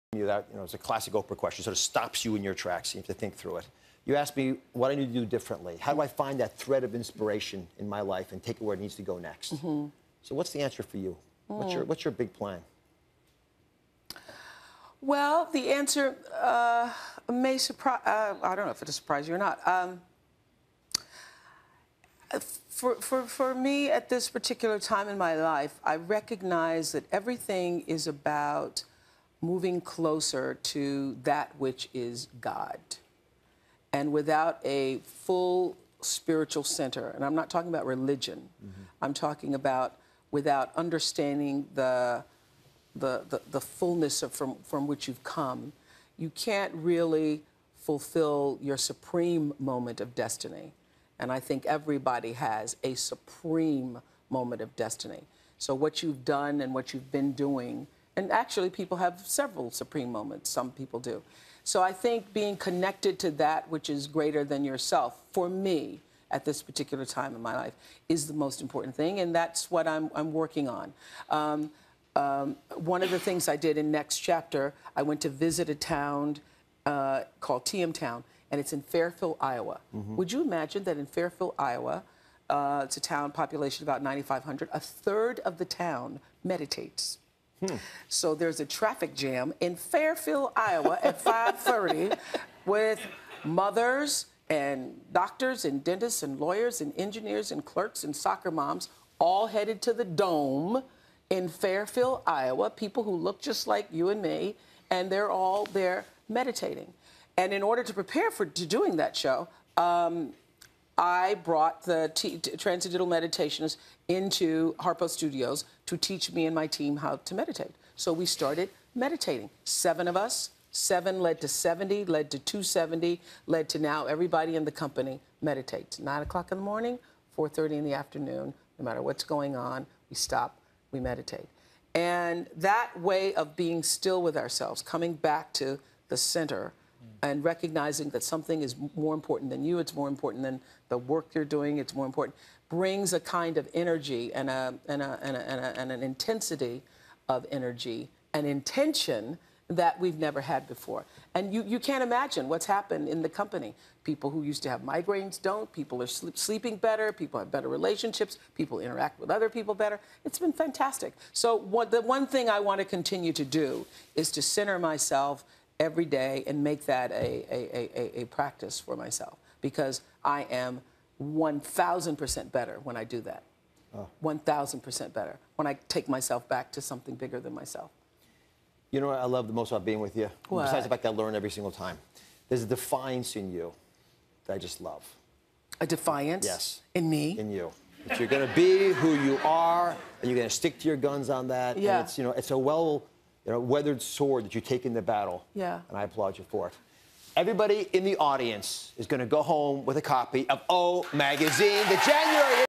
Oprah discusses rush hour in Fairfield, Iowa, TM Town_Opra o Transcendentalnoj meditaciji.wmv
From: Part 7. ) at 1:15--3:00, Oprah discusses rush hour in Fairfield, Iowa, TM Town, where everyone goes to the domes to meditate, how TM benefits her and all her employees who do TM twice a day at work, and the value of taking time for your Self in that way.
OPRA govori o Transcendentalnoj meditaciji: Kako trecina stanovnika u Fierfield-u /Ajova/ odlazi svakodnevno u Zlatne hale da meditira; kako TM tehnika doprinosi njoj kao i svim zaposlenim u njenoj kompaniji koji meditiraju dva puta dnevno i o vrednosti odvajanja vremena za sebe.